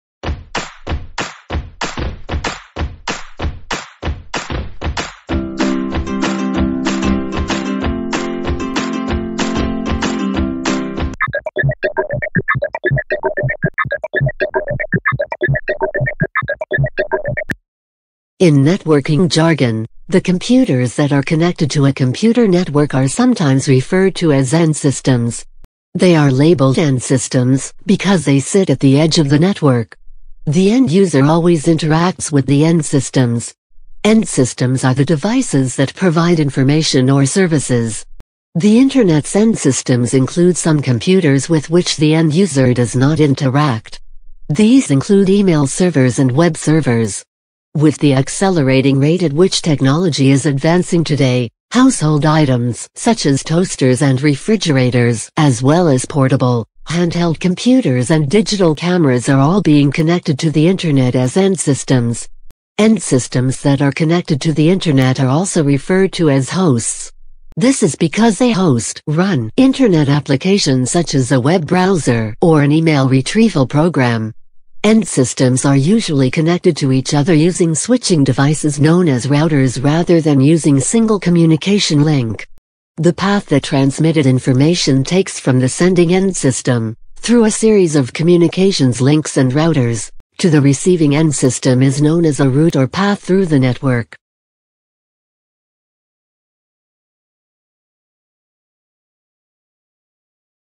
What is END SYSTEM
In networking jargon, the computers that are connected to a computer network are sometimes referred to as end systems. They are labeled end systems because they sit at the edge of the network. The end user always interacts with the end systems. End systems are the devices that provide information or services